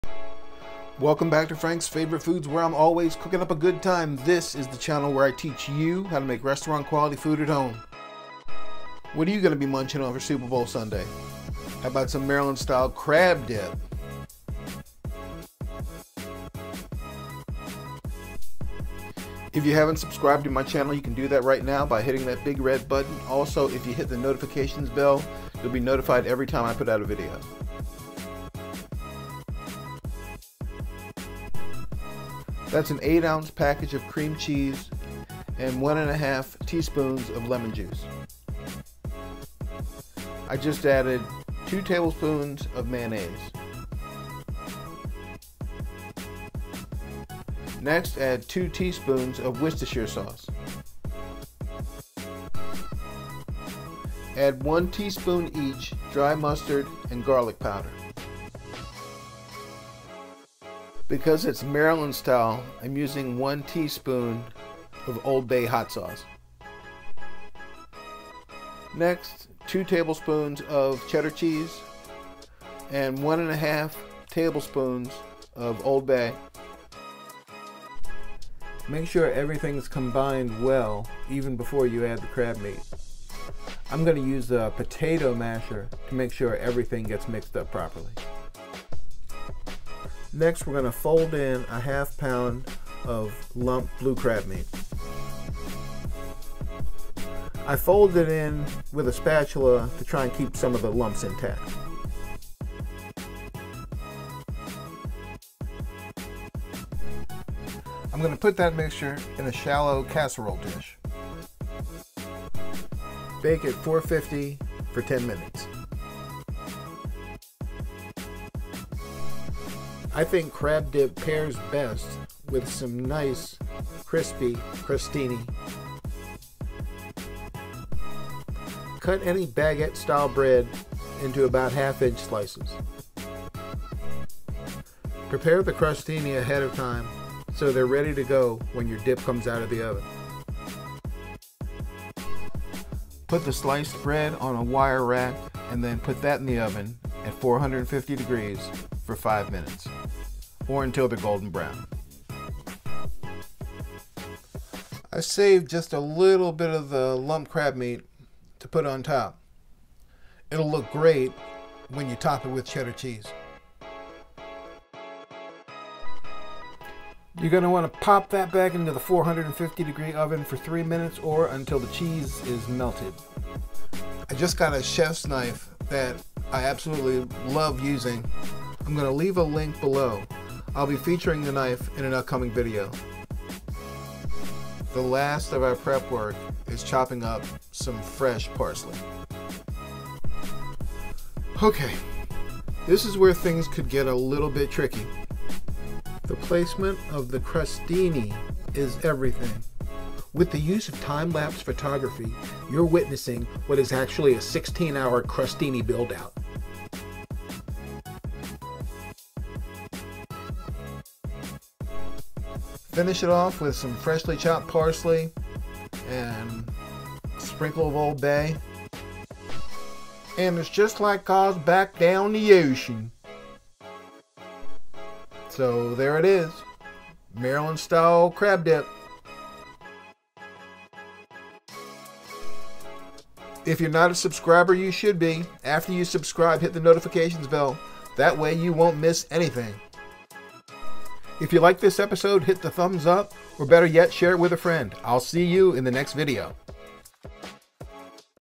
Maryland Style Crab Dip
Maryland Style Crab Dip, chef Frank shows you how to make this crowd pleasing appetizer.

INGREDIENTS
1 8oz. Package cream cheese
2 TBSP mayo
1 1/2 tsp fresh lemon juice
2 tsp Worcestershire
1 tsp dry mustard
1 tsp garlic powder
1/2 cup shredded cheddar
1 tsp old bay hot sauce

1/2 pound blue crab meat
Reserve at little crab meat for topping.

Recipe
1. Combine all ingredients except crab.
(use 2 TBSP cheddar)
2. Cut baguette into 1/2 inch pieces. Bake at 450 for 5 minutes or until brown.
3. Bake crab mix for 10 minutes at 450.
4. Top with remaining crab.
5. Cover with cheese and bake til
cheese is melted.
5. Serve with crostini.

Global Chef's Knife

Old Bay Hot Sauce

Phillips Blue Crab Meat